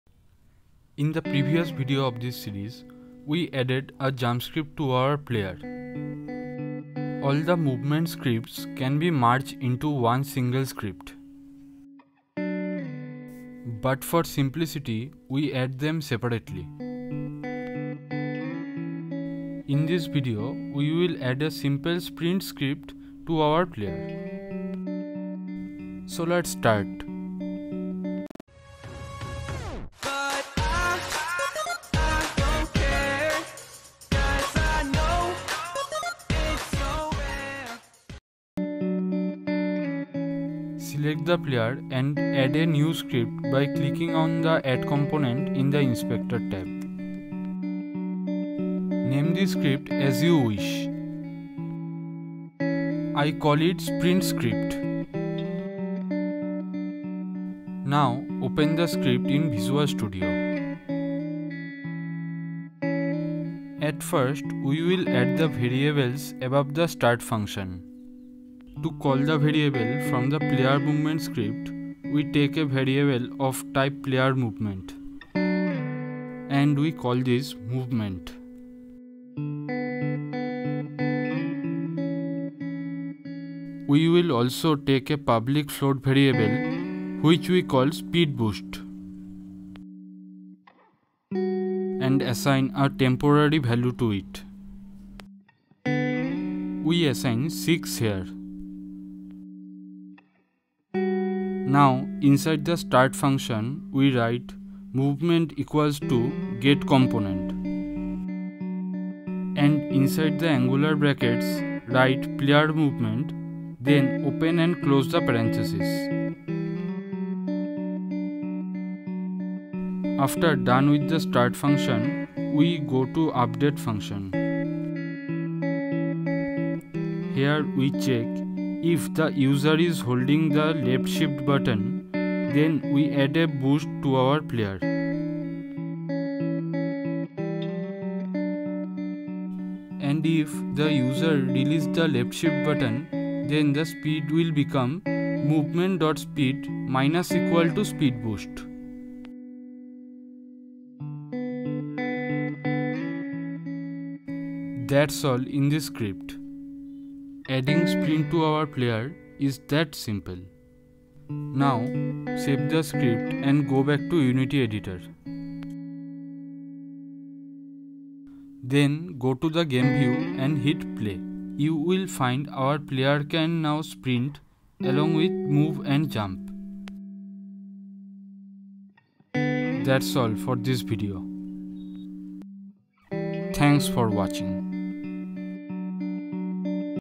Add a Sprint Script to Your FPS Character
This is the fourth video of the fps series, where we will make a complete guide for the first person shooter game.

In this video we add a sprint script to our fps player.